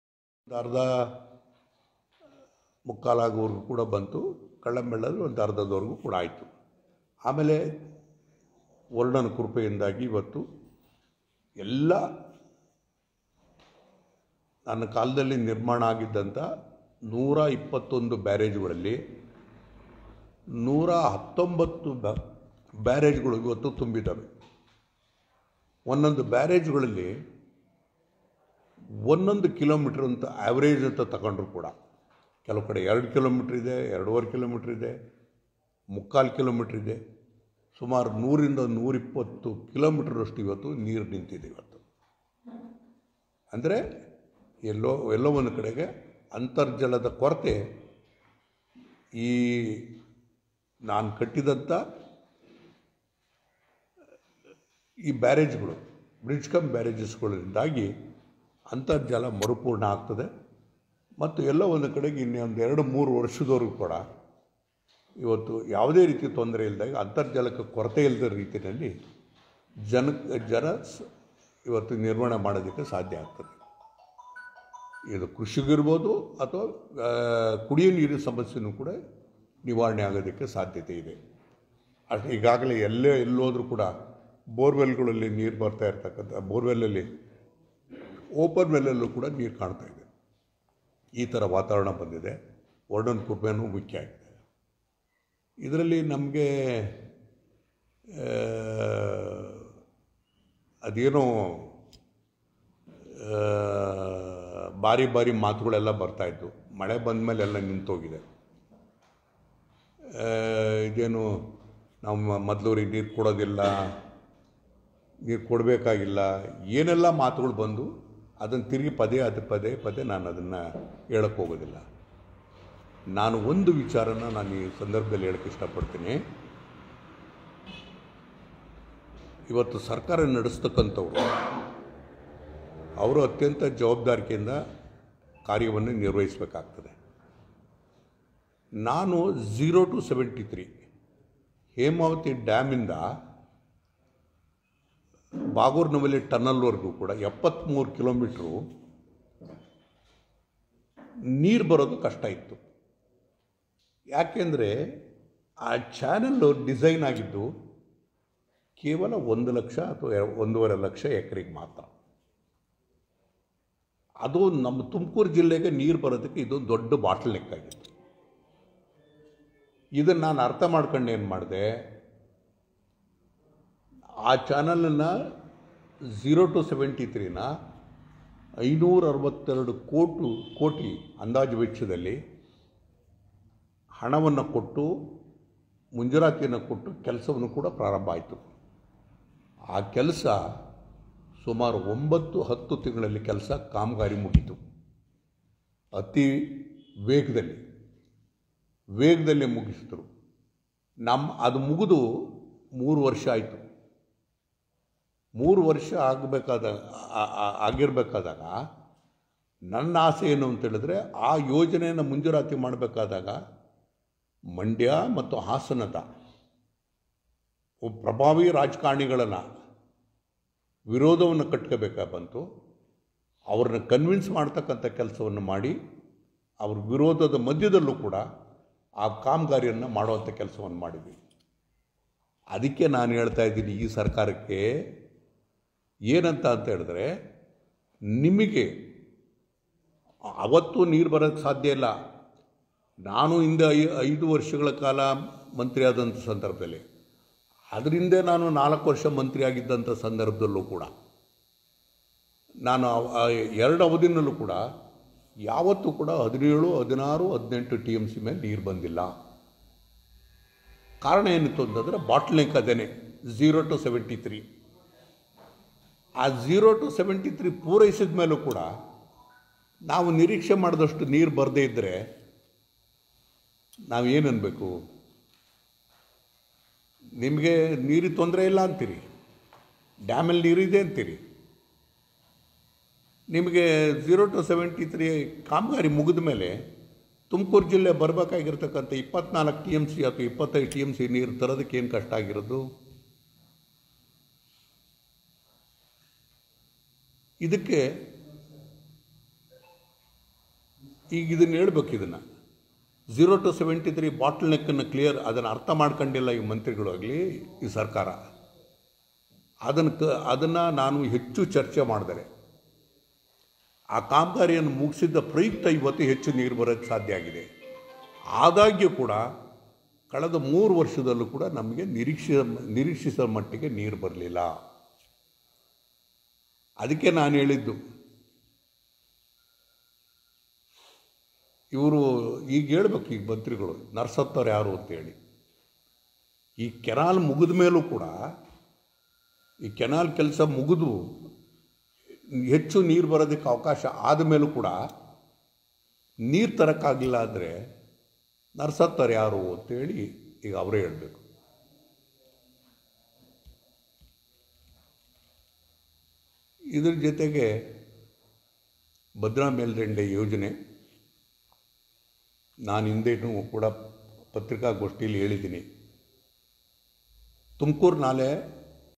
Sri Tv News 4-11-2021
ಶಿರಾ ತಾಲೂಕಿನಾದ್ಯಂತ 119 ಬ್ಯಾರೇಜ್ ಗಳು ತುಂಬಿರುವುದರಿಂದ ಮಾಜಿ ಸಚಿವ ಟಿ.ಬಿ.ಜಯಚಂದ್ರ ನವರು ಹರ್ಷ ವ್ಯಕ್ತಪಡಿಸಿದರು...